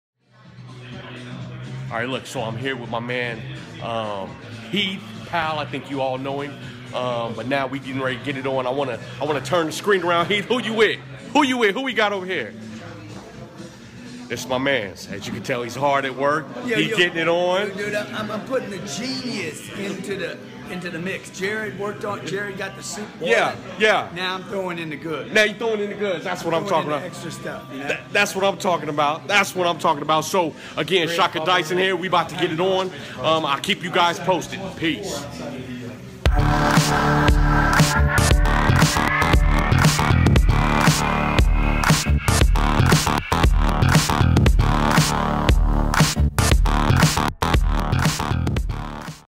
Automotive Sales Expert Grant Cardone with F&I Trainer Shaka Dyson
Grant Cardone discusses bring the Genius with Automotive F&I Trainer Shaka Dyson